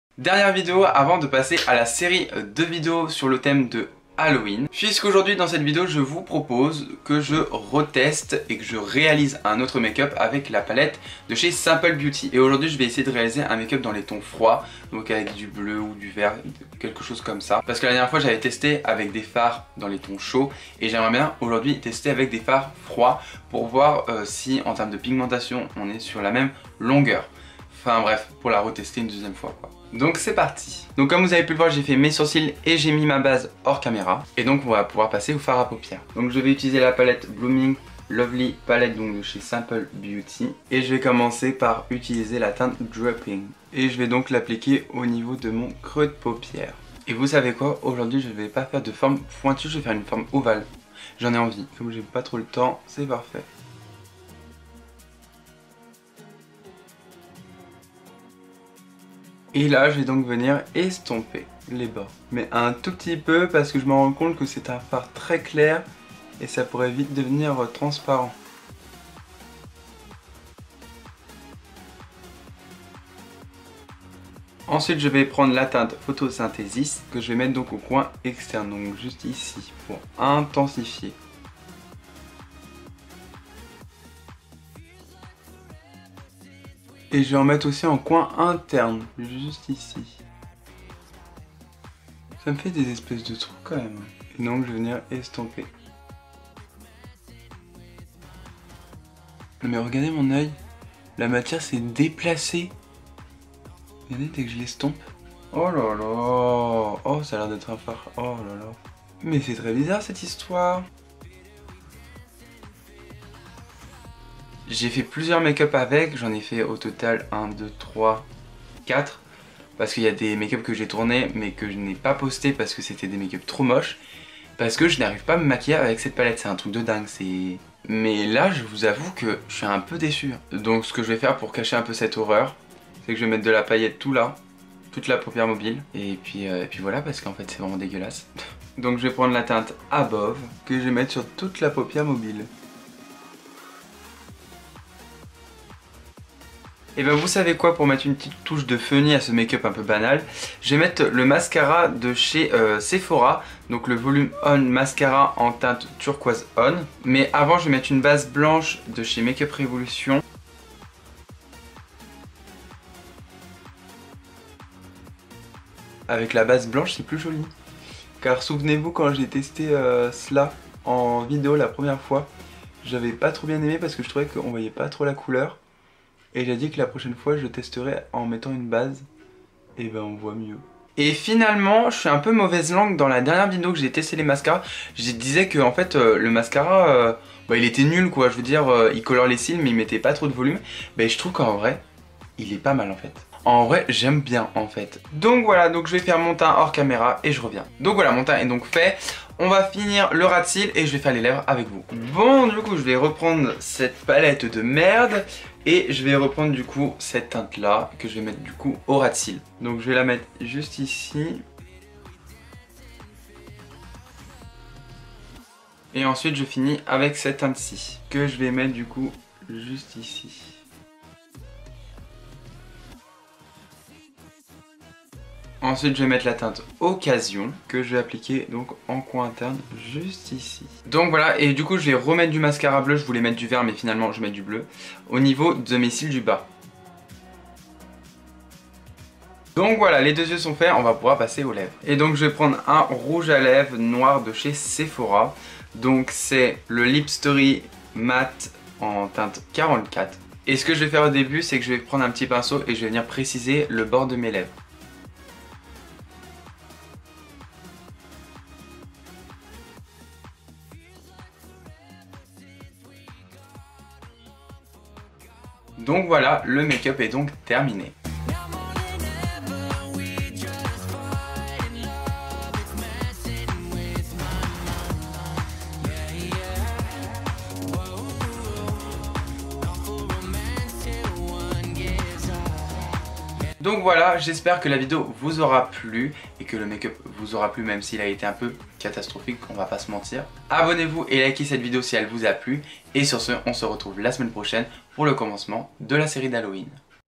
Un makeup HORRIBLE ( Palette de MERDE !! )
Dans cette vidéo, je vous propose un makeup VRAIMENT HORRIBLE, le pire makeup que j'ai pue faire de ma vie ! a cause d'une palette de merde, comment vous dire que j'ai le seum !!
JE VOUS DECONSEILLE D'ACHETER CETTE PALETTE !